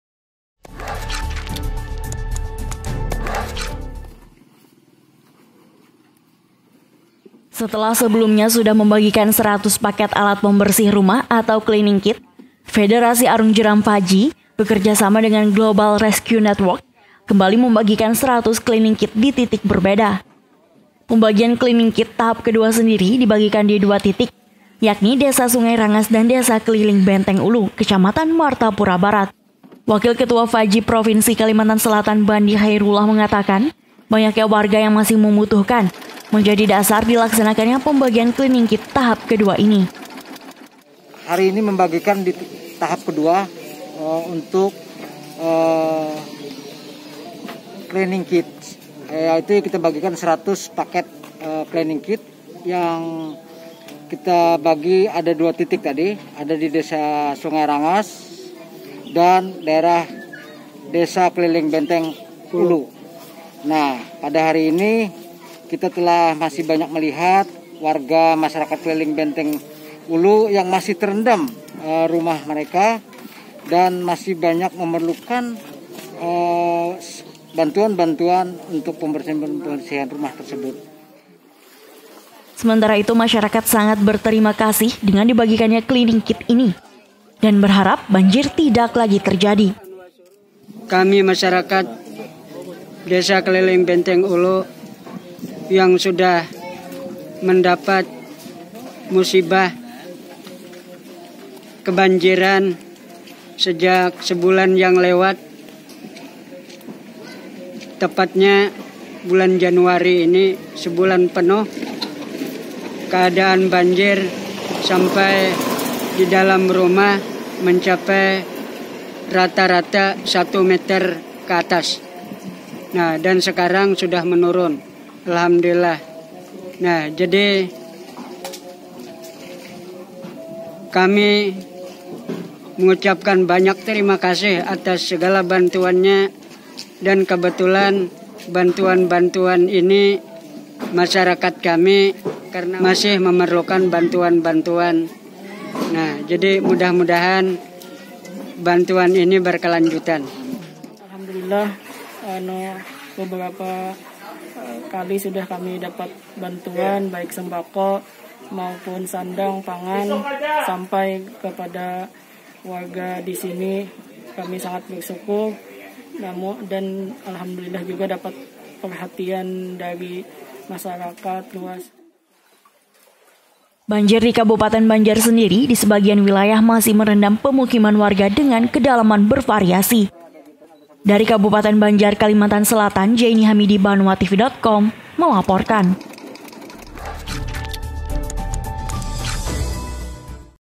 Cleaning Kit Dibagikan, Warga Masih Membutuhkan
Setelah sebelumnya membagikan bantuan tahap pertama, Federasi Arung Jeram Indonesia bekerjasama dengan Global Rescue Network Jakarta, kembali membagikan alat pembersih rumah atau Cleaning Kit ke titik lain di Kabupaten Banjar.

Tahap kedua ini sebanyak ratusan paket Cleaning Kit dibagikan sebagai bentuk meringankan beban warga terdampak banjir di pasca banjir yang melanda Kabupaten Banjar.